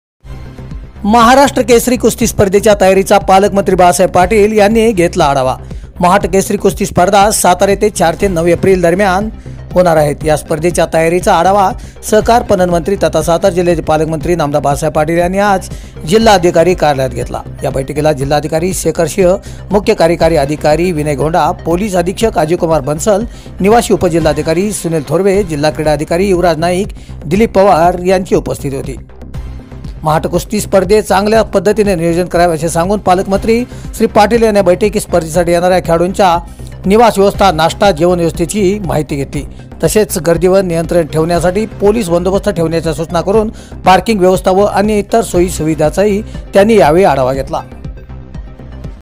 महाराष्ट्र केसरी कुस्ती स्पर्धेच्या तयारीचा पालकमंत्री बाळासाहेब पाटील यांनी घेतला आढावा
सातारा येथे  4 ते 9 एप्रिल दरम्यान होणार महाराष्ट्र केसरी कुस्ती स्पर्धा